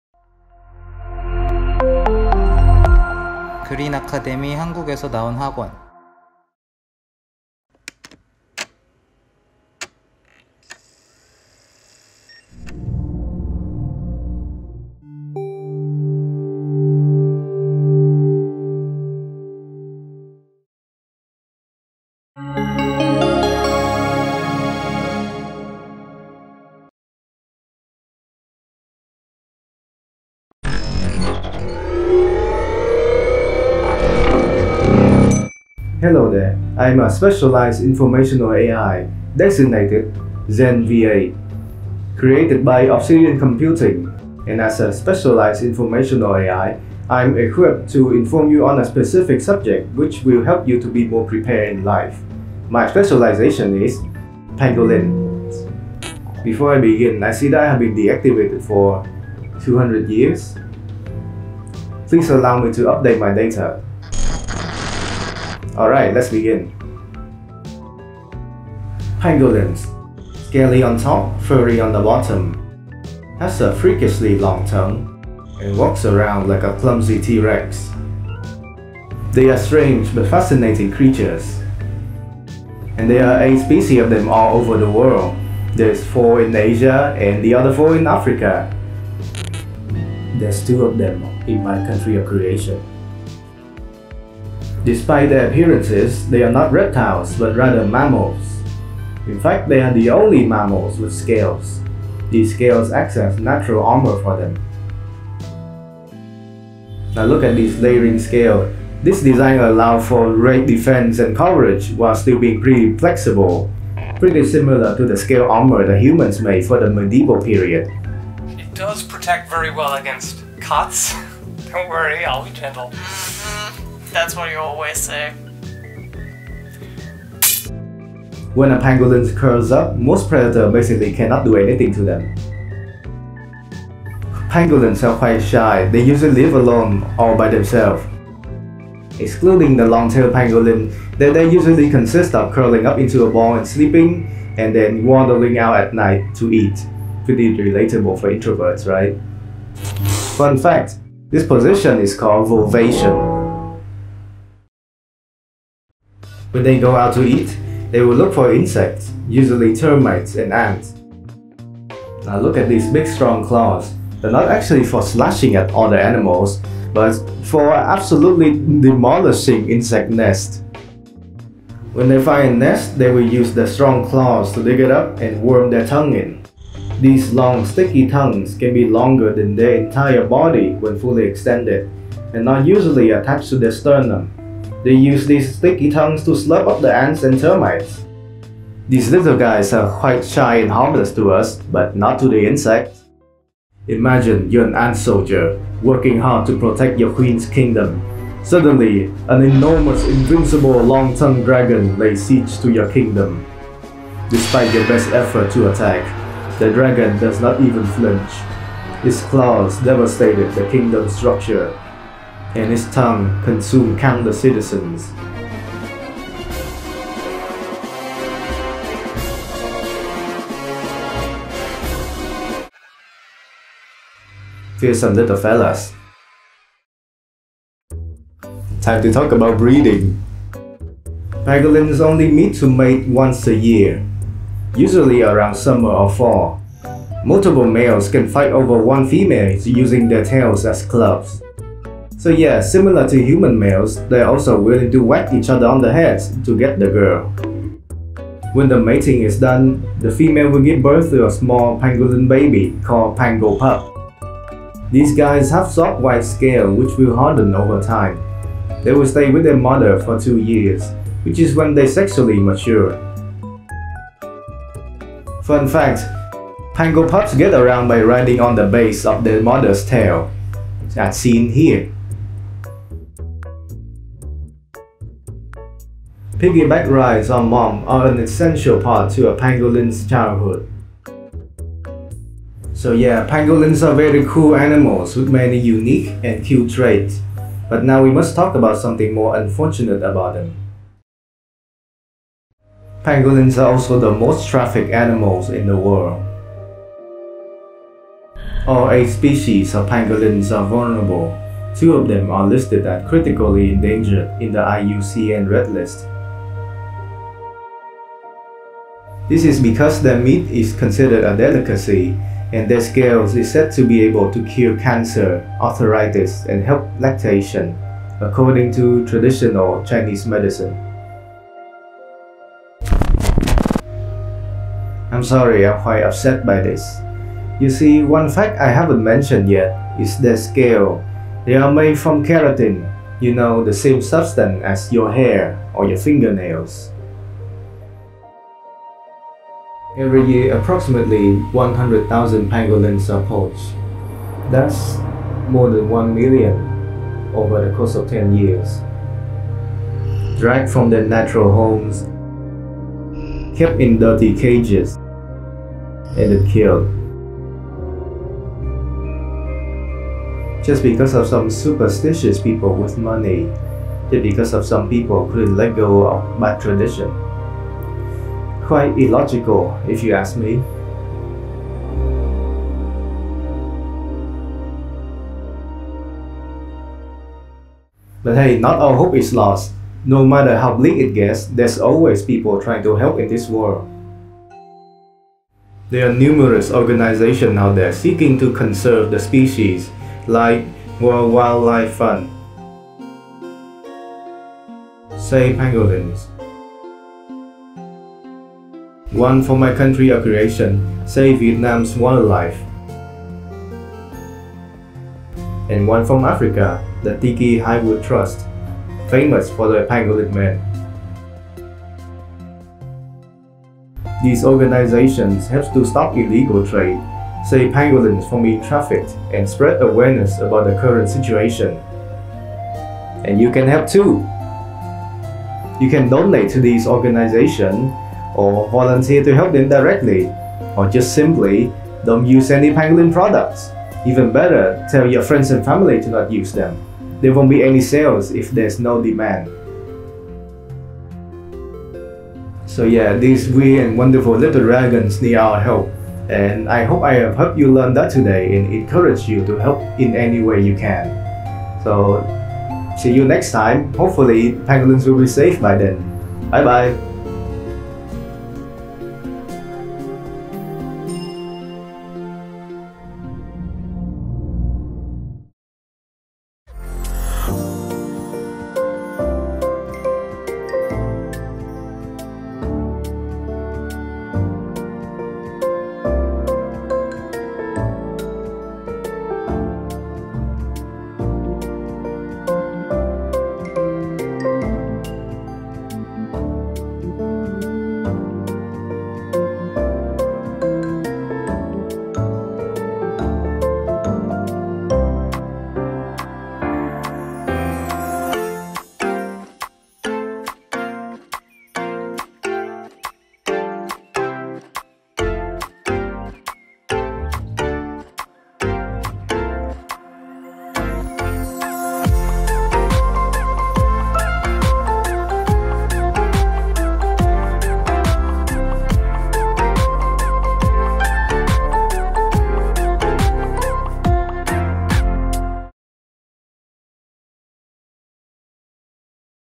Pangolins: Tiny Long-tongued Dragons (A science doc I made for my Cinematography/Editing Class)
While others in my class were making adverts, I was studying the pangolins. I hope you'll enjoy this strange solo project of mine. Now that i've finished this month long project, i'll have time to do my usual gaming vids!

Credits:
Directed by
Bùi Nguyễn Hoàng Hải

Art and Assets by
Edra Ridho

Actors
Bùi Nguyễn Hoàng Hải… .Zen
Pangolins… .Themselves